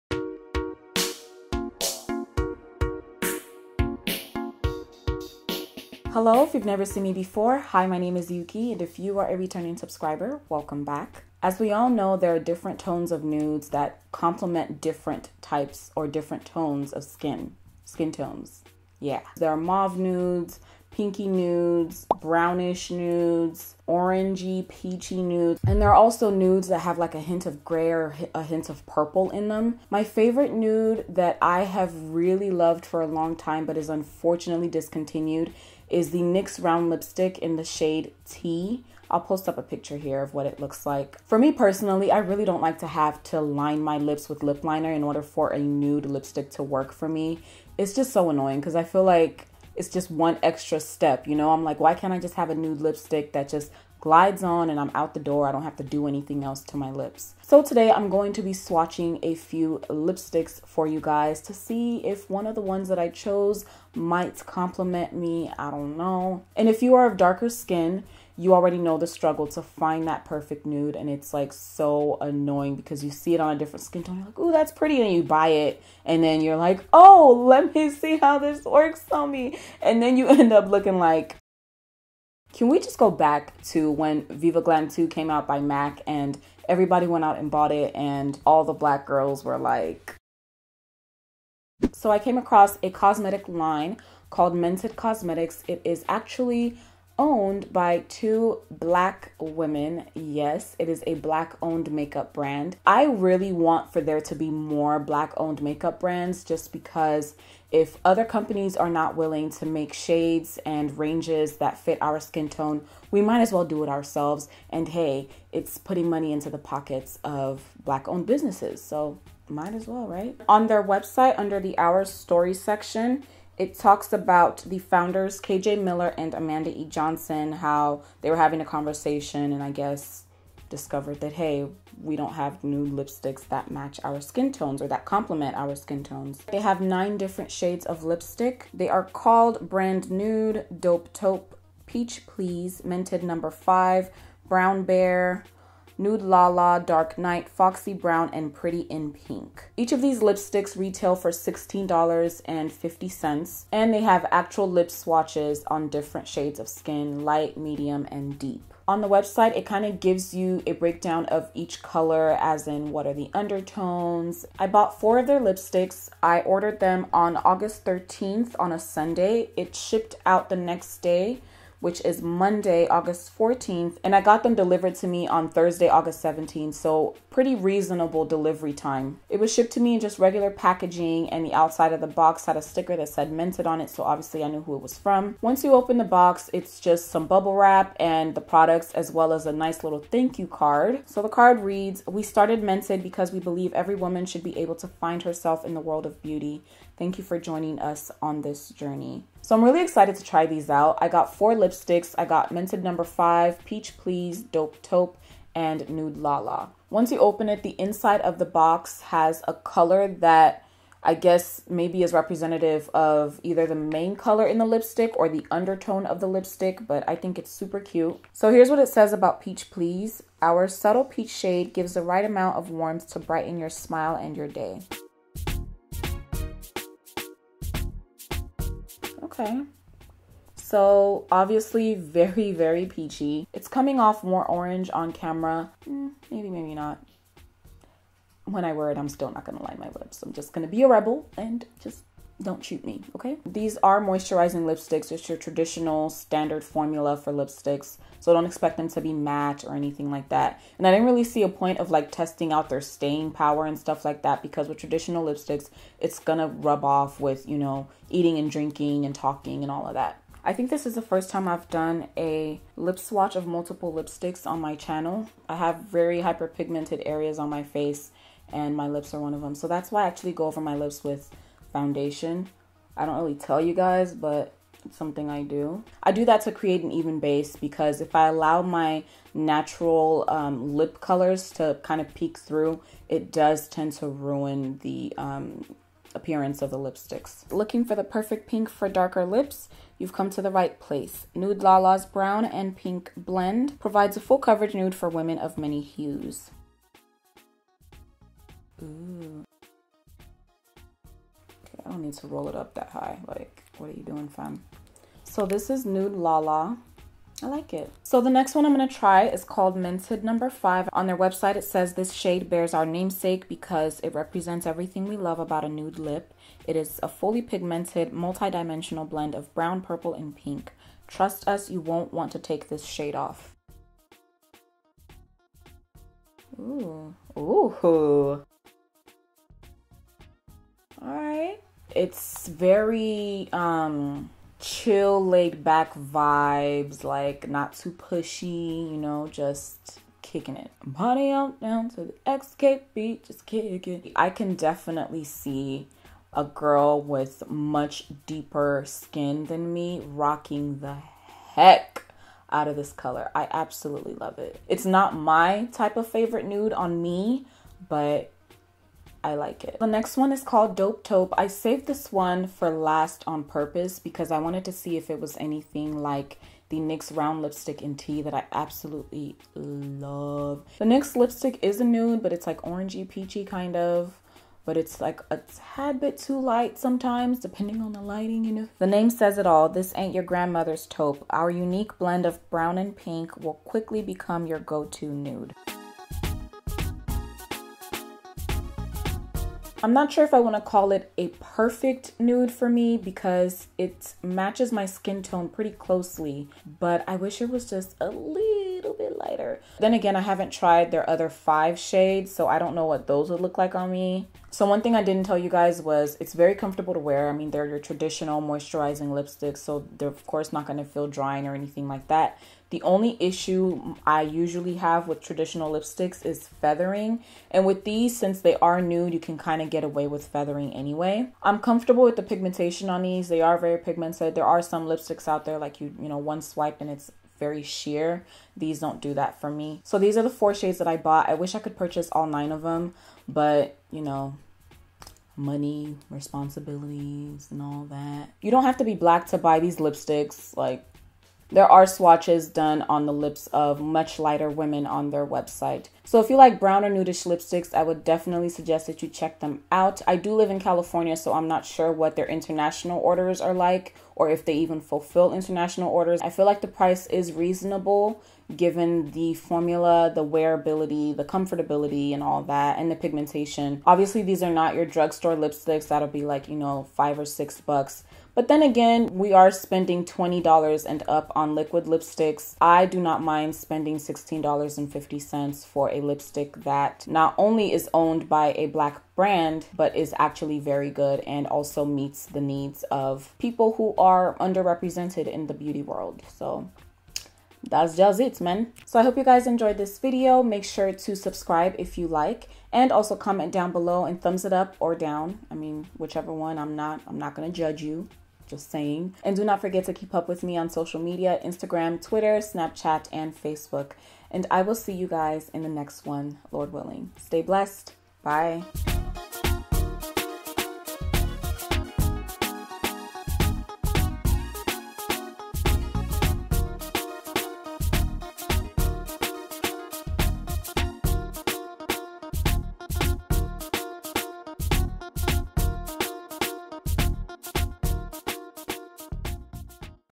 Trying Nude Lipsticks by Black Owned Makeup Brands 😳
Vero: Youkeyy

Youkeyy here!  So I thought I would get on the Black Owned Makeup Brand (B.O.M.B) video hype.  But this time instead of a black owned makeup tutorial, I thought I would try out nude lipsticks since the struggle to find the perfect nude lipsticks for dark skin and brown skin are very real.  You really do not need to be a black woman to understand this struggle as many people with varying skin tones on the darker end of the spectrum may or may not struggle with this as well.  I have been seeing Mented Cosmetics more frequently and decided to try out their lipsticks.  They are a black owned makeup brand that tailors to people who may find it difficult to find that perfect nude lipstick.  I purchased four of their lipsticks from their website and I got Peach Please, Dope Taupe, Mented no 5, and Nude LaLa.  They were $16.50 each and shipping was fast.  Unfortunately the company does not ship internationally as of the date this video is going up but per their website, that will be changing soon.  Hope you enjoy and do not forget to watch in 1080p for the best quality.

Equipment Used for My Videos:
My Camera
My Lighting
My Lens
My Tripod
My Microphone
My Microphone Accessories